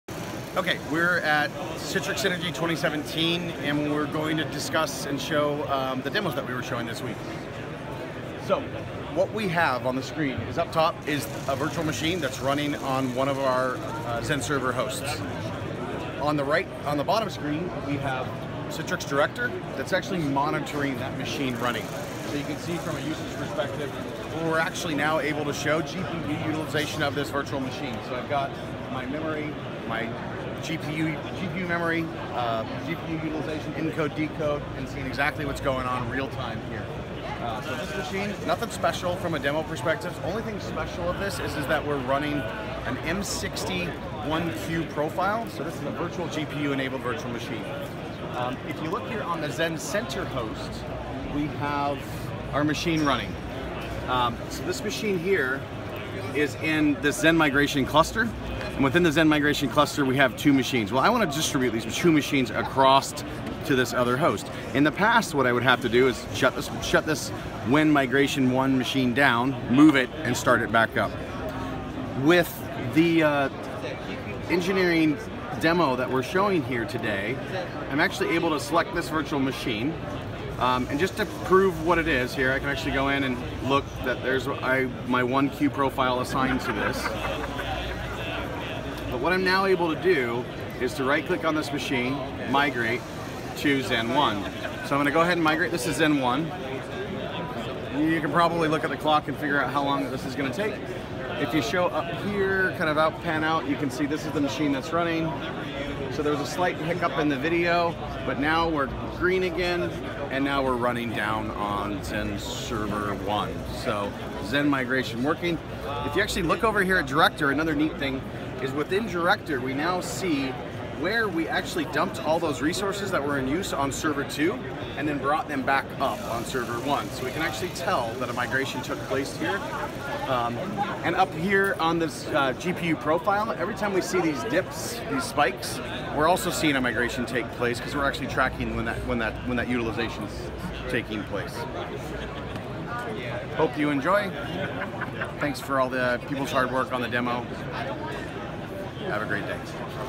NVIDIA GRID Monitoring using Citrix Director & vGPU Live Migration with Citrix XenServer
Jared Cowart, Technical Product Manager at NVIDIA, demonstrates at Citrix Synergy 2017 new NVIDIA GRID features that enable greater insight and management of vGPU in Citrix environment.